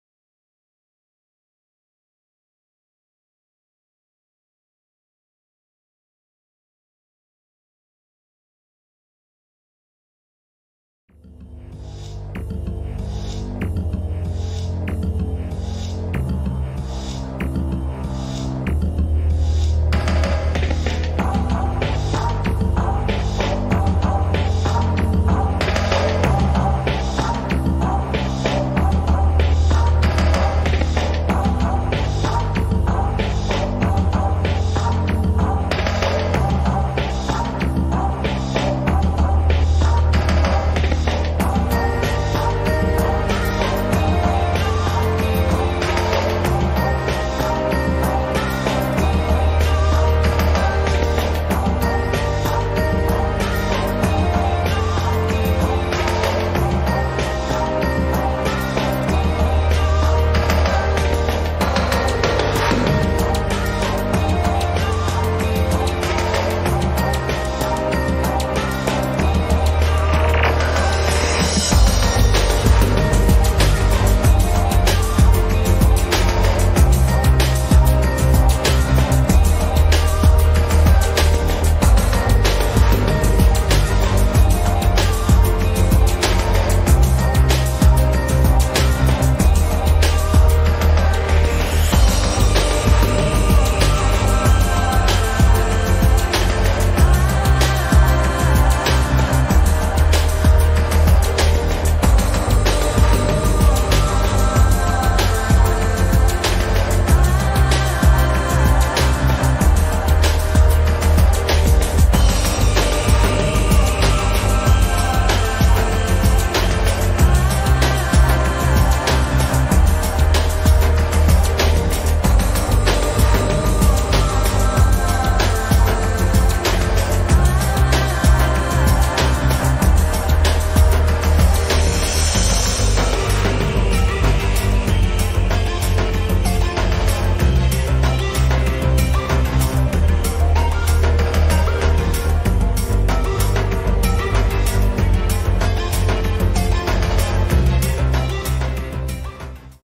11-4-2022 🌩️ Tormenta eléctrica en las inmediaciones al anochecer
WEBCAMS JOSE REY METEO ☀️⛈️☃️🌈⛅. Imágenes y datos meteorológicos actuales desde Villamediana de Iregua (La Rioja, España) Live 24/7.